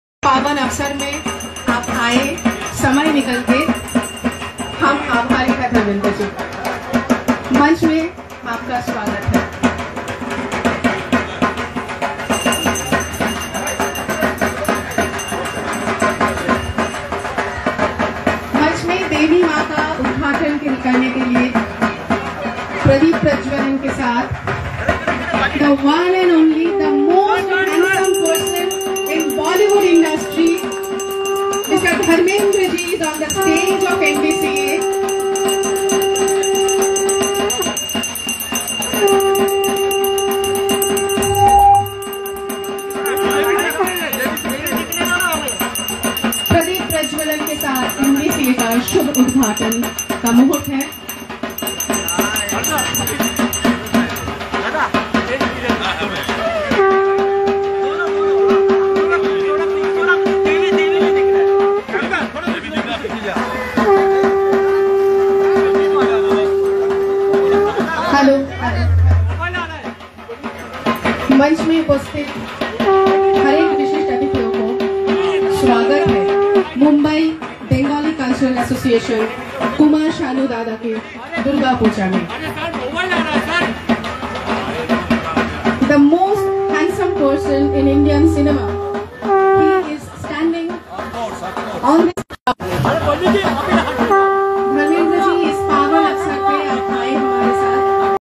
Legendary Dharmendra At Kumar Sanu's Durga Puja Opening Ceremony in Mumbai
Join us as we capture the mesmerizing moments from the star-studded Durga Puja Opening Ceremony in Mumbai, featuring none other than the legendary Dharmendra. In this exclusive video, witness the grace and grandeur of the event, as well as the presence of iconic Bollywood actor Dharmendra alongside the melodious Kumar Sanu. Immerse yourself in the spirit of Durga Puja and experience the glamour and cultural richness of this festival. Don't miss this unforgettable celebration in the heart of Mumbai!